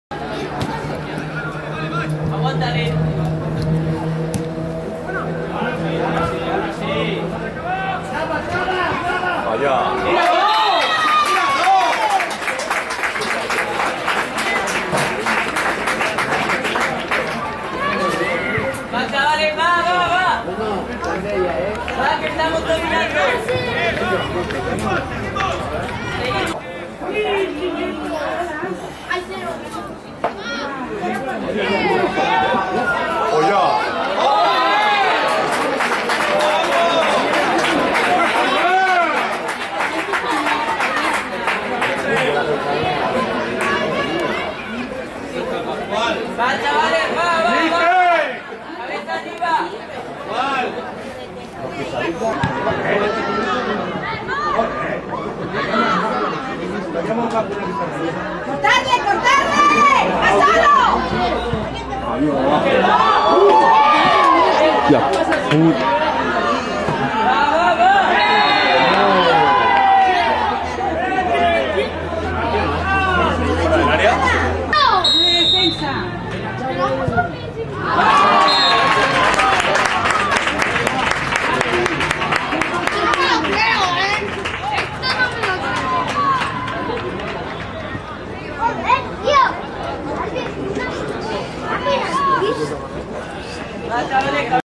2014-15 J18 Sant Joan Despí 1-3 Igualada Resum Gols
Campionat de lliga Infantil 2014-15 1ª Divisió Grup 3 Jornada 18 28/II/2015.
Sant Joan Despí 1-3 Igualada